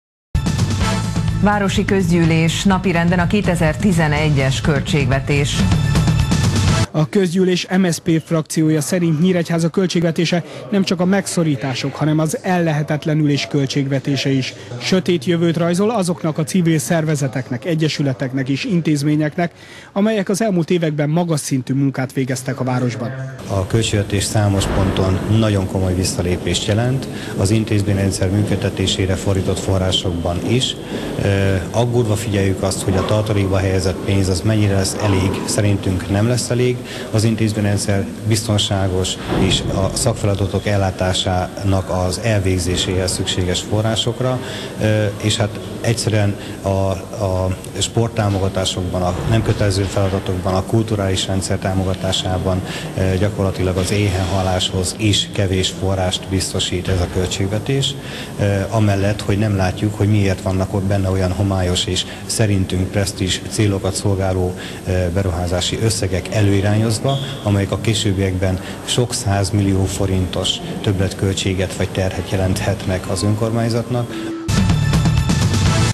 Bizonytalan költségvetés - 2011.02.10. - Nyíregyháza
A költségvetés a megszorítások, az ellehetetlenülés, az átgondolatlanság költségvetése. Köszönőviszonyban sincs az egekig emelt szakértői vizsgálat magasztos megállapításaival, tehát valaki nem mondott igazat akkor, amikor a korábbi tervezés realitását kétségbe vonta. Bizonytalanságot szül a város oktatási, kultúrális és szociális intézményrendszerében, egyszerre üzen hadat és nyújt fenyegetettséget az eddig országosan elismert rendszereknek. A tervezett átalakításokhoz rendelt, tartalékba helyezett összeg megítélésünk szerint nem fogja fedezni a biztonságos működést, a szakfeladatok eddigi magas szinten történő ellátását. Vélhetően elbocsátásokkal, a munkanélküliség növelésével, sok nyíregyházi sorsának megpecsételésével jár majd a hatalmi koncentráció kiépítése érdekében beharangozott átalakítás. Ez történik az országban, ez valósul meg Nyíregyházán is. Nem látható semmilyen prioritás a 2011. évi büdzsében, ugyanakkor példátlan és felfoghatatlan az, hogy olyan homályos és gyanús, sok százmilliós, több évre áthúzódó tételeket tartalmaz, amelyekről a közgyűlésnek semmilyen ismerete nincs, ennek megfelelően még csak előzetes döntést sem hozott.
Fűnyíróelv alapján vesz el minden szférából pénzeket, több esetben sújtja az elesetteket, a nehéz helyzetbe jutottakat, szociális érzéketlenség süt belőle. Nyíregyháza hanyatlásának költségvetését nem tudjuk támogatni.